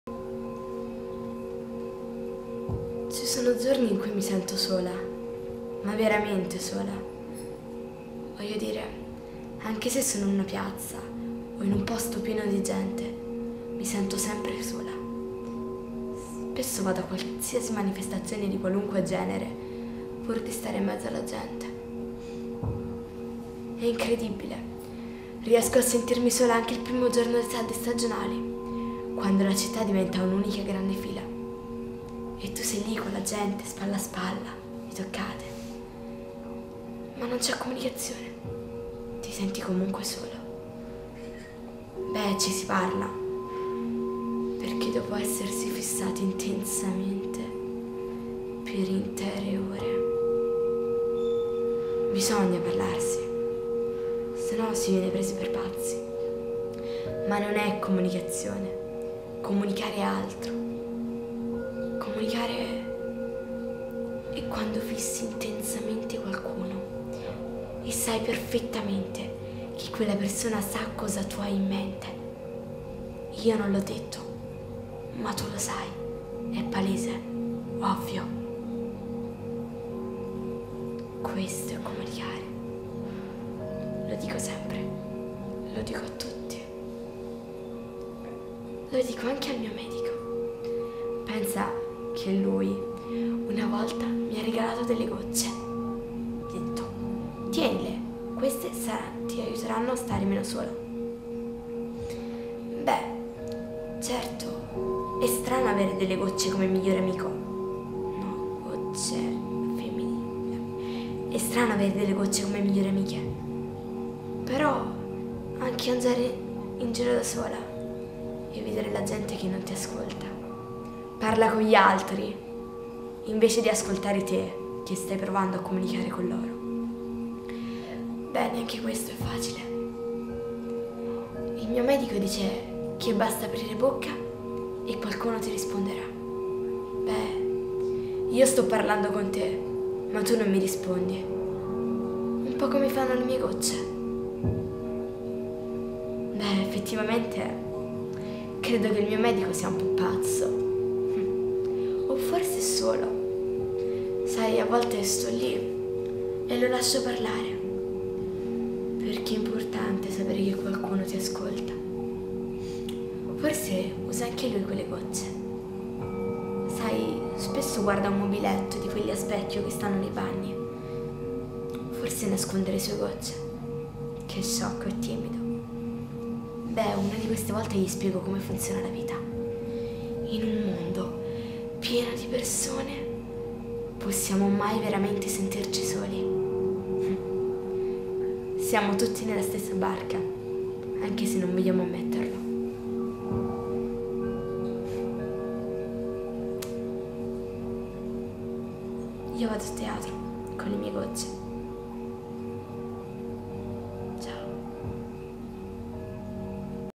EG CHILD ACTORS LAB - Sarah Nettis 3
EG SHORT monologo con Sarah Nettis, realizzato durante l'EG CHILD TRAINING ACTORS LAB del Centro Internazionale Studi Espressività Generativa (tm) . Acting coaching e Regia di Gianluca Testa. Centro Internazionale Studi Espressività Generativa (tm) .Contatta Sarah Nettis per il tuo film.